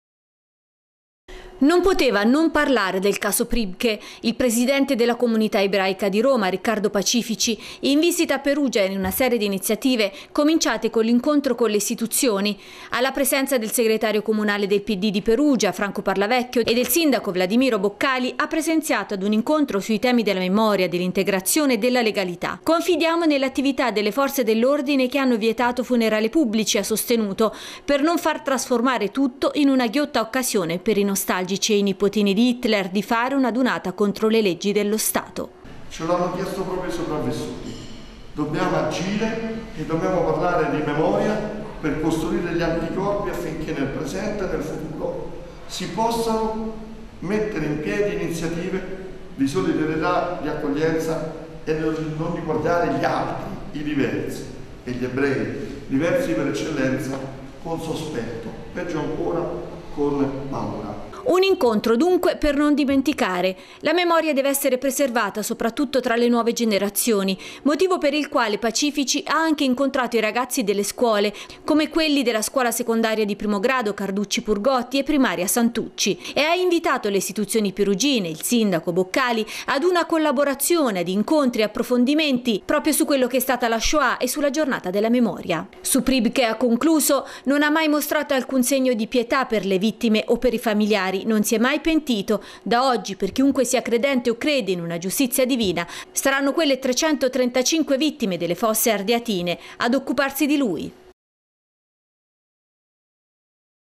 L'Umbria "apre" ad Israele
Riccardo Pacifici: «Perugia capitale della cultura ebraica nel 2014»